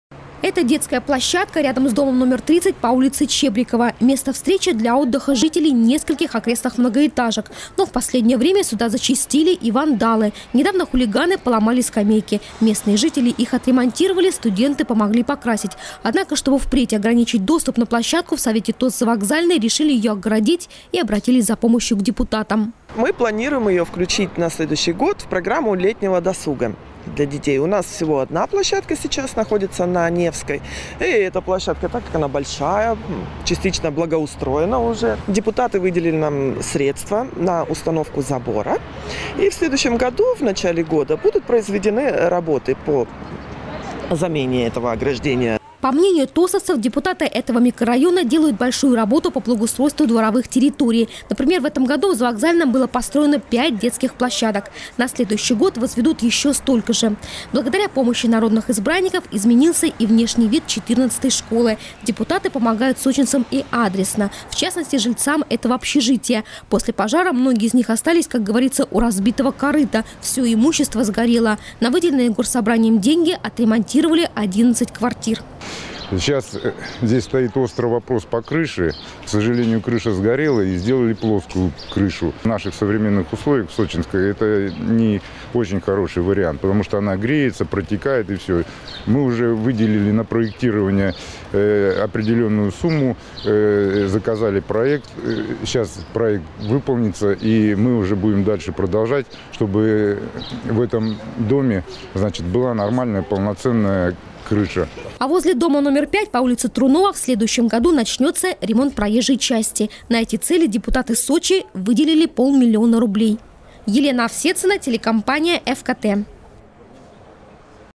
В микрорайоне Ареда детских и спортивных площадок станет больше. Новости 24 Сочи
Люди устали чинить качели и скамейки после того, как по ночам на площадке по своему "отдохнут" хулиганы. За помощью представители совета ТОС «Завокзальный» обратились к депутатам. Во время встречи договорились — народные избранники сочинцам помогут. Кроме того, стало известно, что в следующем году в микрорайоне детских и спортивных площадок станет больше.
Сочи. Новости.
Телеканал Эфкате.